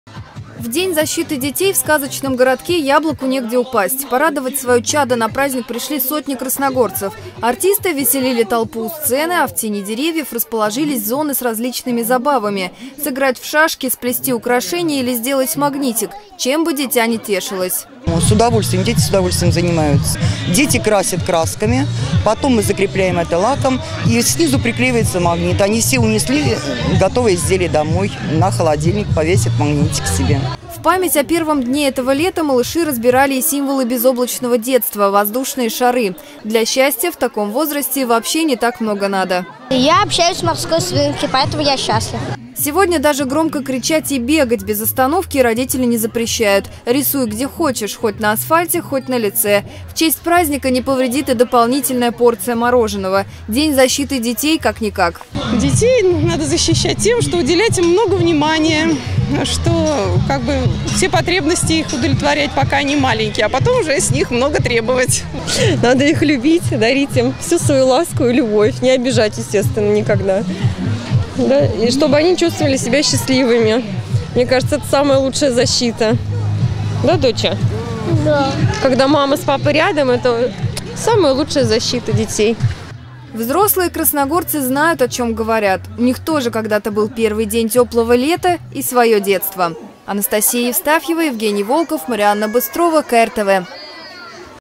КРТВ День защиты детей
Эфир от 2 июня 2014

День защиты детей — один из самых старых международных праздников. Маленькие красногорцы все выходные отмечали его  в детском городке «Сказочный». Для малышей была развернута большая программа с играми, мастер-классами и шоу-номерами.